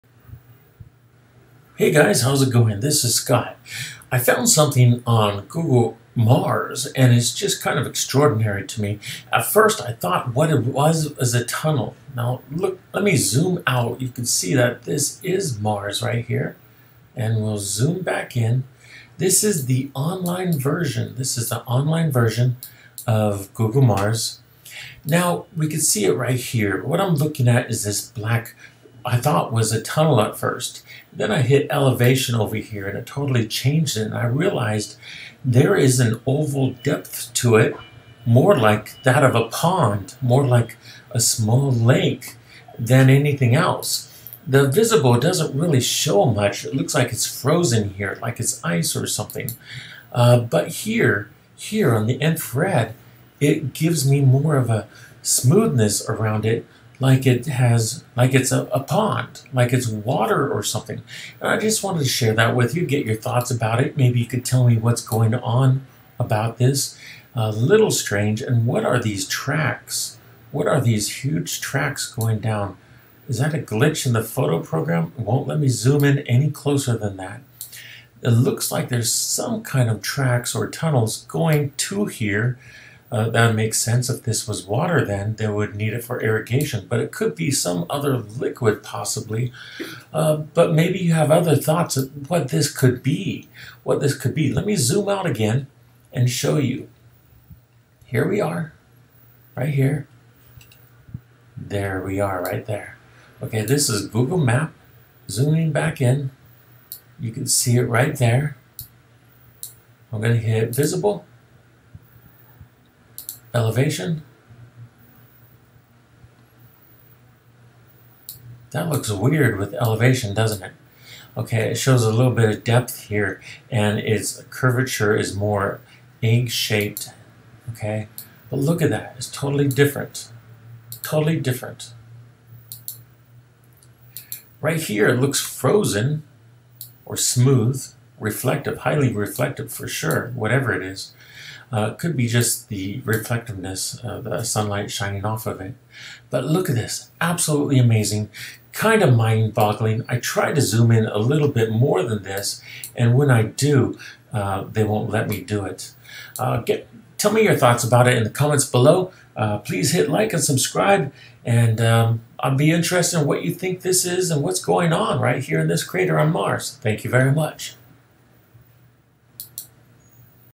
I Found A Pond In A Crater On Mars, Google Map Discovery, UFO Sighting News.
Date of discovery: Dec 16, 2021
Location of discovery: Mars

First off, when you click the URL above it takes you to the online Google Mars map, but you will need to hit the button in the upper left corner that says infrared.

I found a pond in the center of a crater on Mars using google earth map. The pond clearly looks like a liquid and and is even on all sides of it. Its not a tunnel or a cave, but an area covered in a liquid. It could be water, but may be some there liquid entirely. I know its not a tunnel entrance because if I hit the button in the upper left that says Elevation, you can see that its depth is small, oval and similar to that of a pond. I just wanted to share this with you. Water on Mars, we all know it exists, but if this is water, its a lot. Over 3km across!
Scott C. Waring - Taiwan